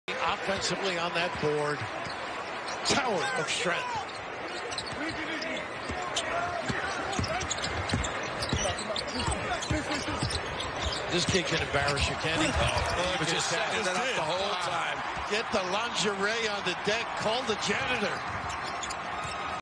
Davion Mitchell Incredible Move Against Gonzaga
Davion Mitchell had 15 points, 6 rebounds, and 5 assists including this nasty double in & out to blow by Aaron Cook in Baylor's Championship win against Gonzaga last night.